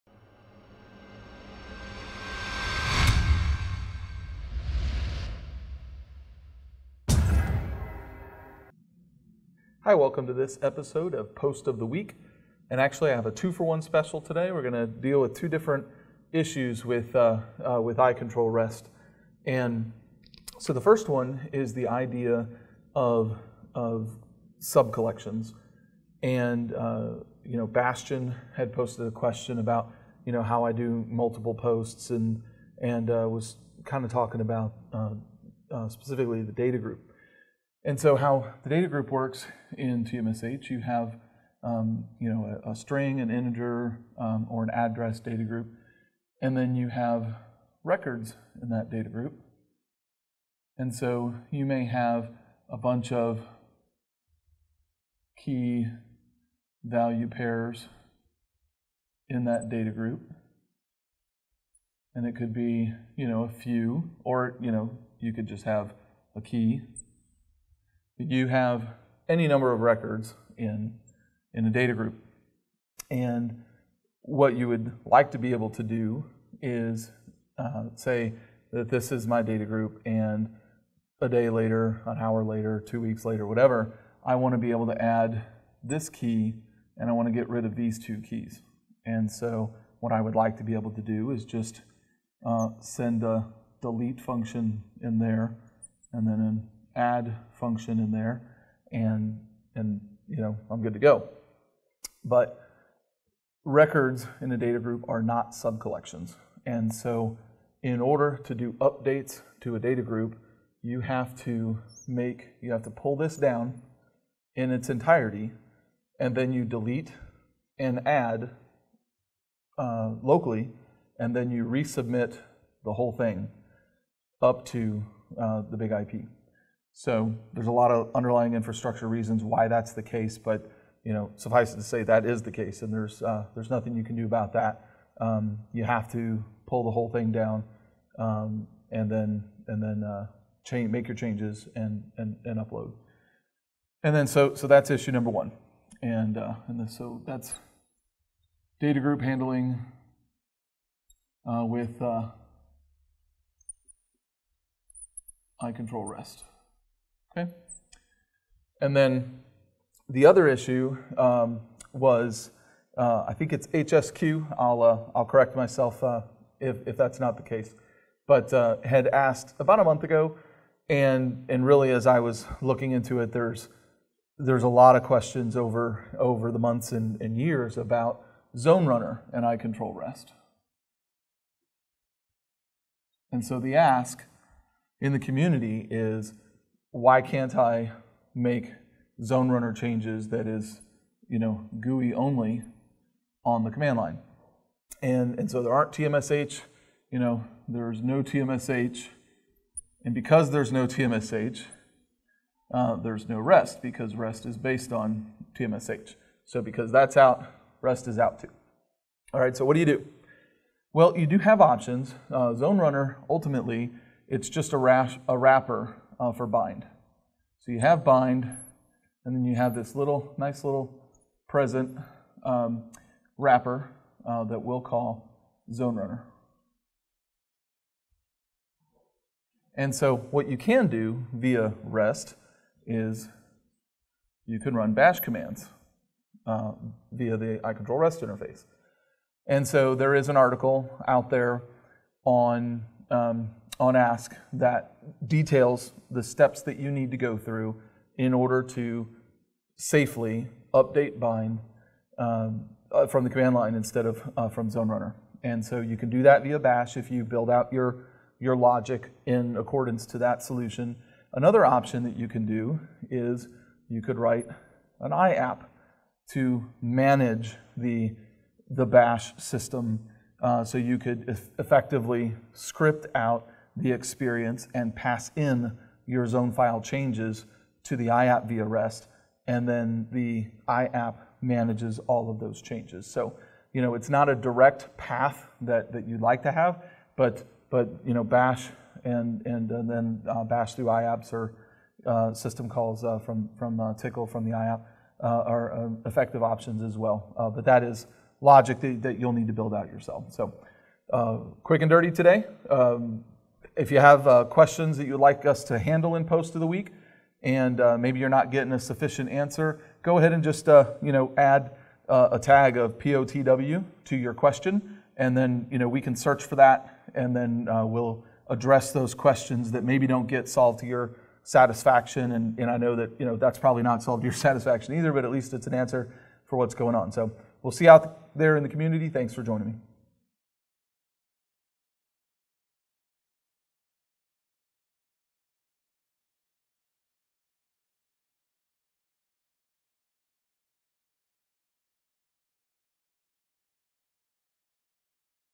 Post of the Week: iControl REST Subcollections & ZoneRunner Options
In this episode of Post of the Week, we explore two key topics: iControl REST subcollections and ZoneRunner options. The discussion focuses on the functionality of subcollections within iControl REST, highlighting their significance and application. Additionally, we delve into various ZoneRunner options, providing insights into their configuration and usage. Join us for a comprehensive overview of these important features.